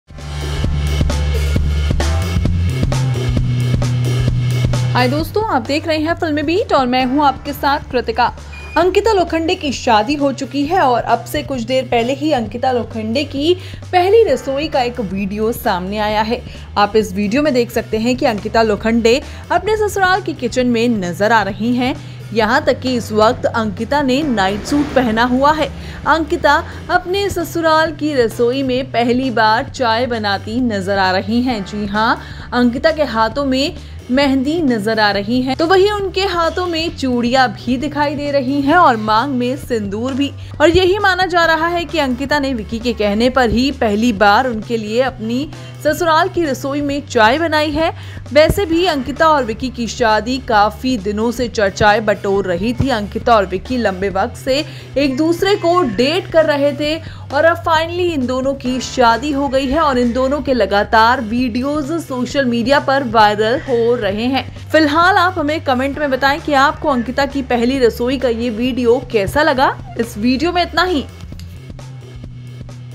Ankita Lokhande ने ससुराल में पहली बार Vicky Jain के लिए ऐसे बनाई चाय; Video | FilmiBeat
Ankita Lokhande's first rasoi rasam video from sasural kitchen will make you happy  . Watch the video to know more about this !